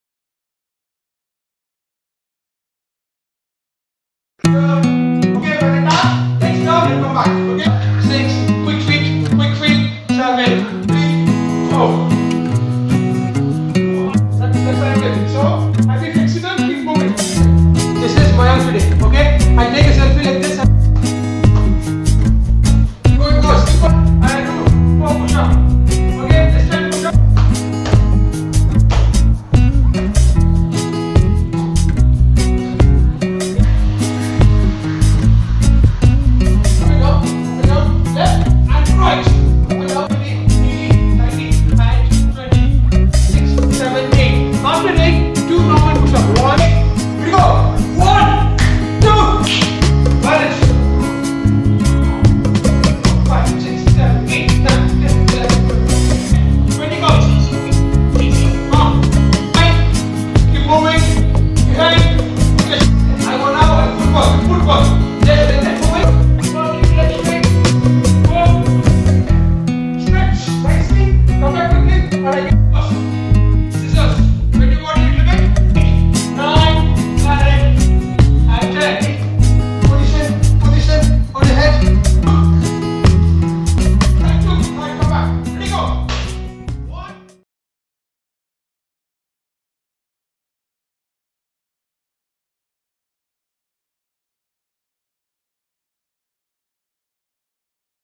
BTS OF SPORTHEADS ONLINE SESSIONS WITH COACH JAY
SPORTHEADS ™ Presents

Take a sneak peek into how our online sessions are conducted by @jayavandan10 (head coach) with the technical support of @sportheads.in

During this quarantine, it is the whole team's effort to come back on the pitch stronger, hence we are making the most out of this quarantine.

Minor setback for a major comeback!